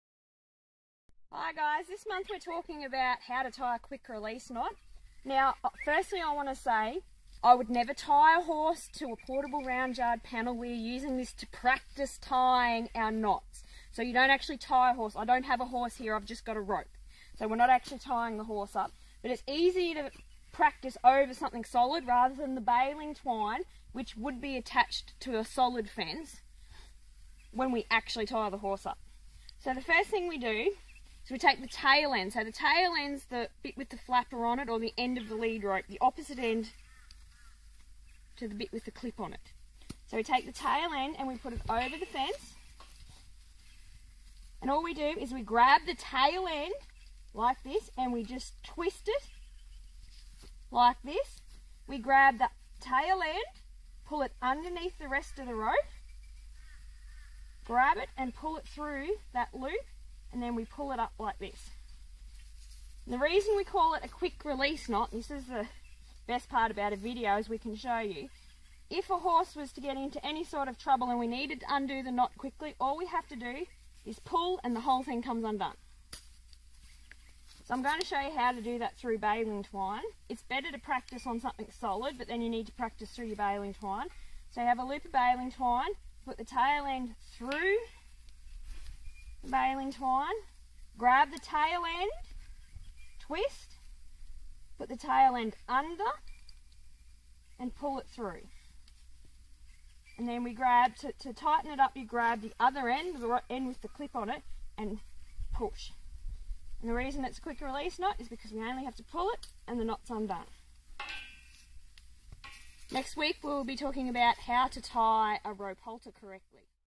How To Tie A Quick Release Knot - Local Horse Magazine
A demonstration of how to tie a quick release knot - For full article check out Local Horse Magazine a FREE online monthly